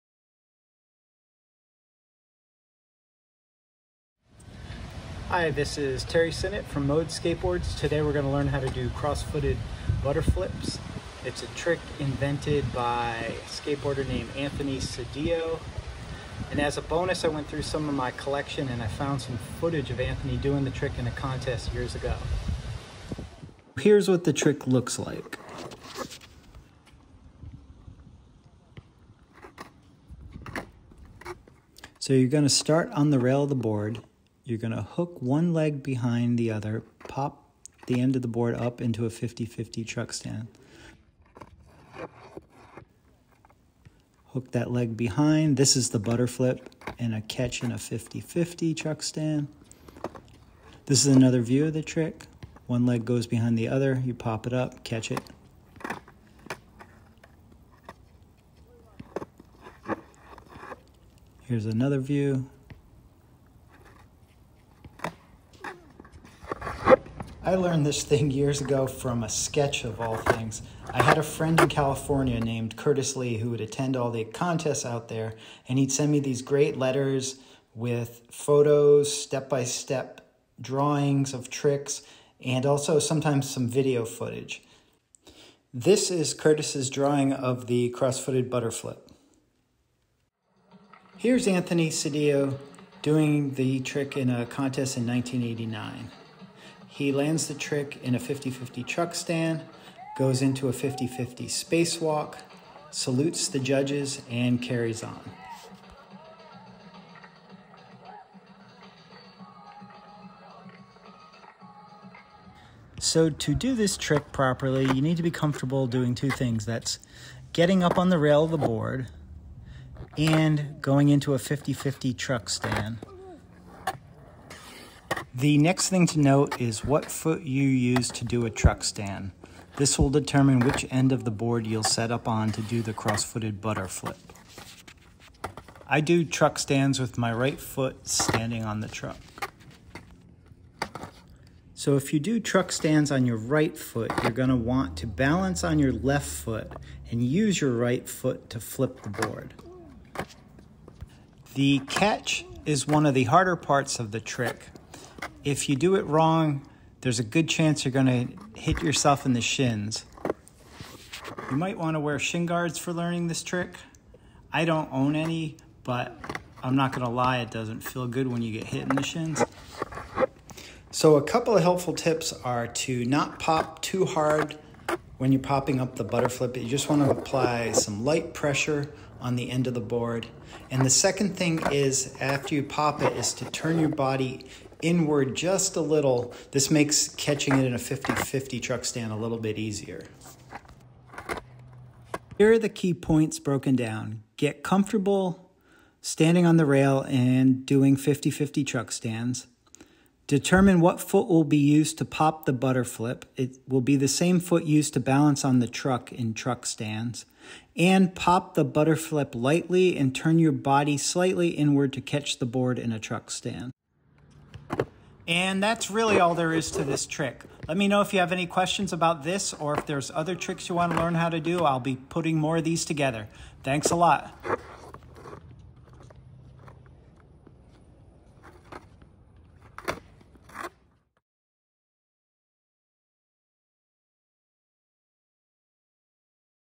Freestyle Skateboard Trick Tip: Cross-footed Butterflips
In this freestyle trick tip, learn how to do cross-footed butterflips with Terry Synnott of Mode Skateboards. We also share some history of the trick and some classic footage of its originator, Anthony Sedillo. Enjoy the video and let us know if you have any questions or suggestions for future trick-tip videos.

In a future video, we will delve into the traditional butterflip, as created by Keith Butterfield. This one focuses on a variation.

Credits:
Curtis Lee skate clip: Filmed by Susan Trimble, and distributed by John Trimble.

And, one clarification, the Anthony Sedillo footage is from Phoenix, not Tempe, AZ.